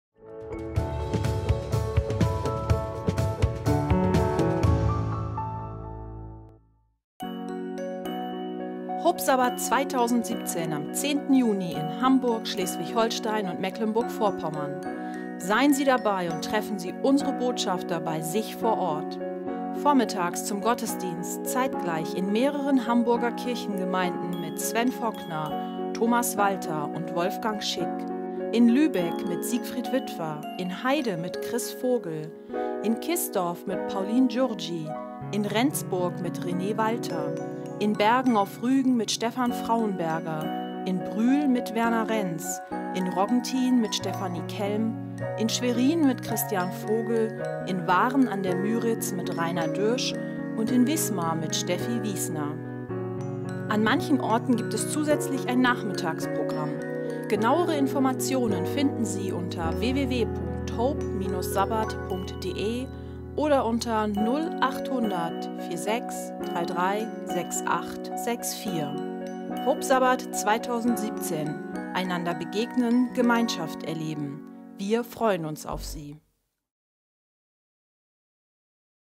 Hope Sabbat 2017 – Einander begegnen – Gemeinschaft erleben!
Am 10. Juni in Hamburg, Schleswig-Holstein und Mecklenburg-Vorpommern.
Dabei sein und das Hope-Team treffen. Vormittags zum Predigtgottesdienst zeitgleich in verschiedenen Städten.